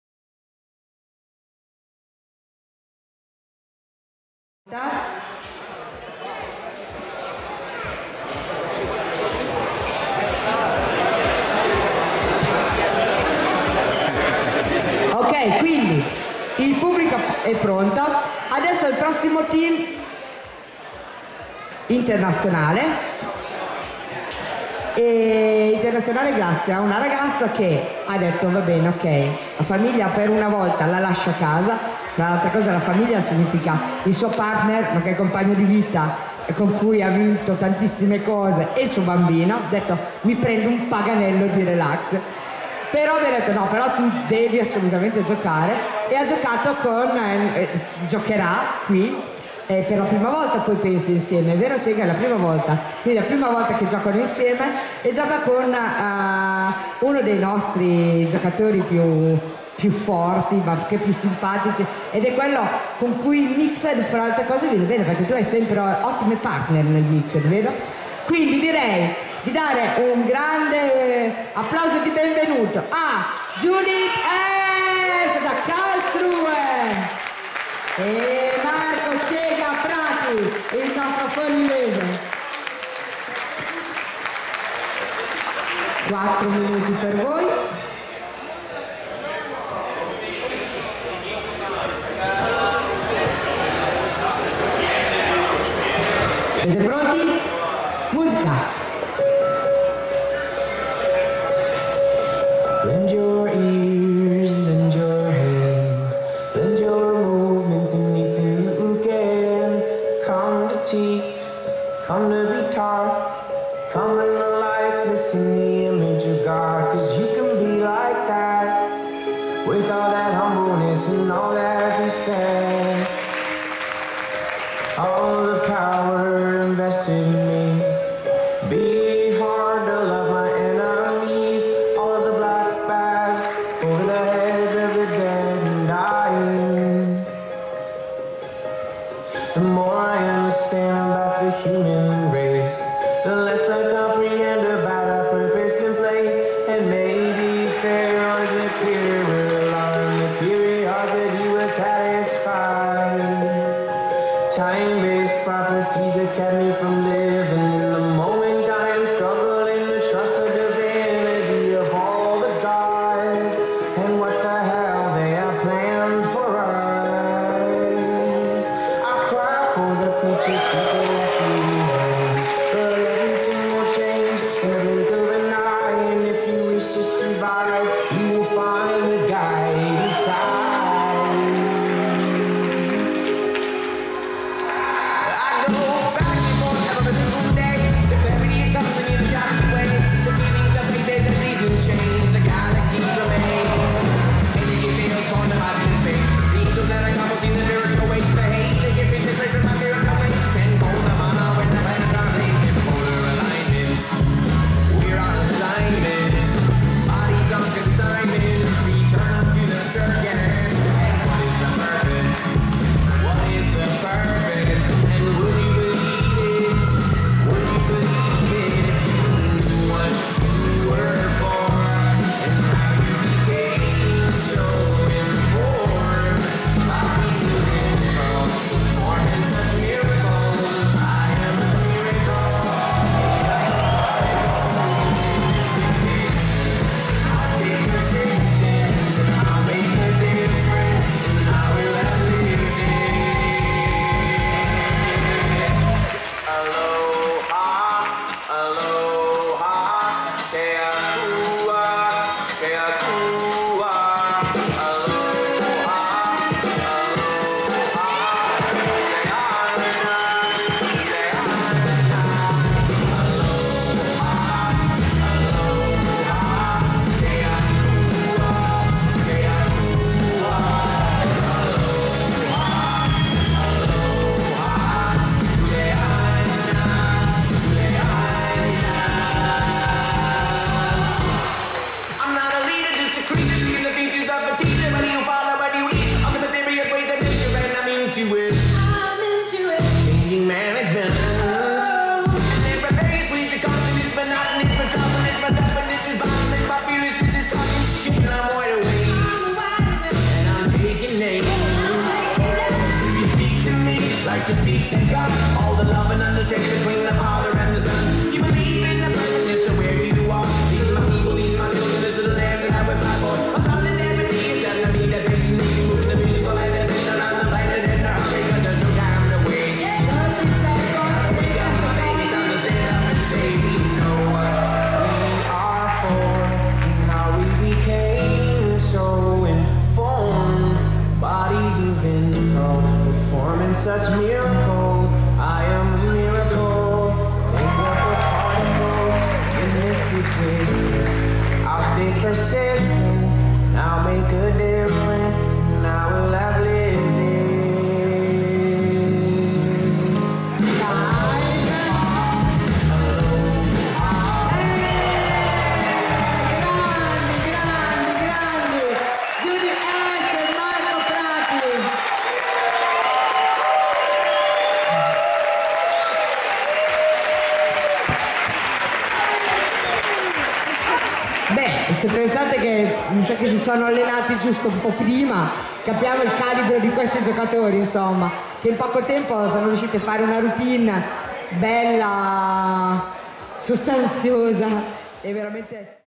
1. Mixed Finals - Judith Hess/Marco Prati
Paganello
Freestyle Challenge 2014
1. Judith Hess/Marco Prati (43.0: A 12.8, E 18.2, D 12.0)